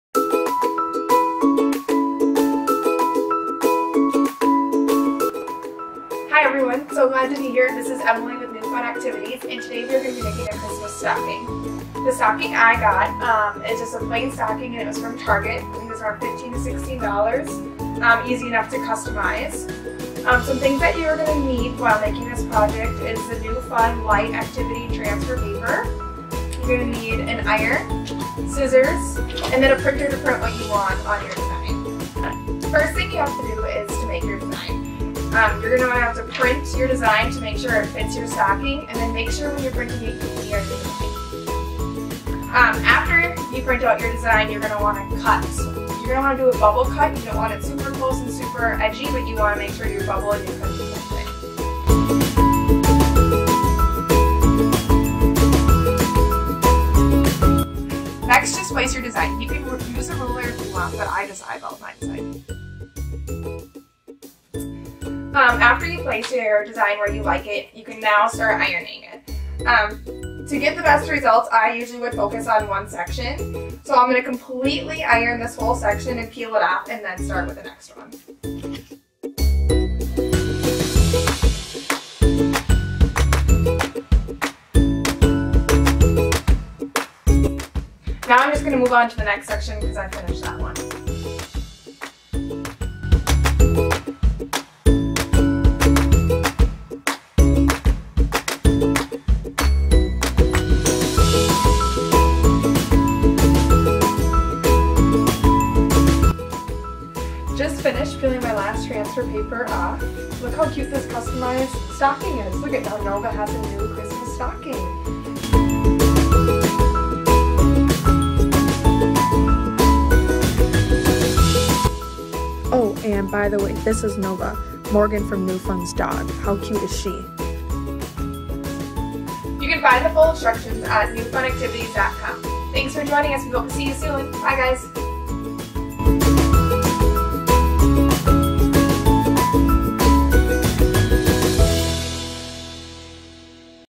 DIY Custom Holiday Stocking
Make your own custom stocking with NuFun Activities! This video will take you step by step on exactly how to create it!  Be sure to check out NuFun Activities Instagram, Facebook and Blog!